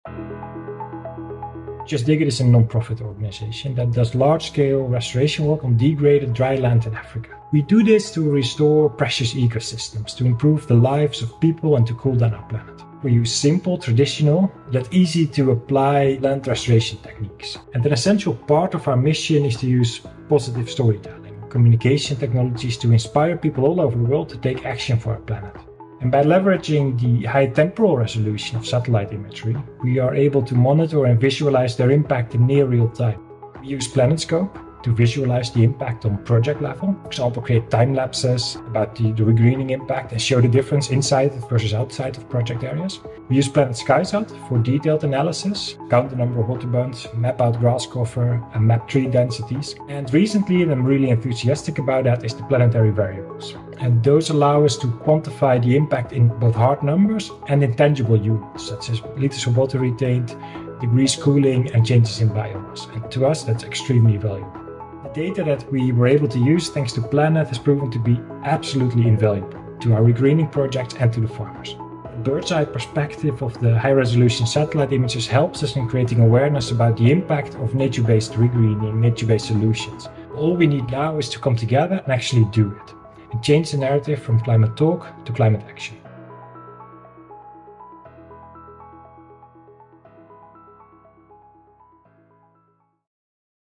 Seeing African Restoration From Space: JustDiggit Leverages Planet Data to Regreen the Land
This video interviews Sander De Haas, Landscape Restoration & Innovation Expert, from the non-profit JustDiggit. Leveraging Planet’s SkySat, PlanetScope, and Planetary Variables products, JustDiggit is leading community-based restoration projects across sub-saharan Africa. In Tanzania, for example, JustDiggit and local communities have dug semi-circle bunds in the ground to catch rainwater and support the re-growth of native grasses. Planet’s data helps document the regreening and changes in the soil moisture and temperature of the restoration sites.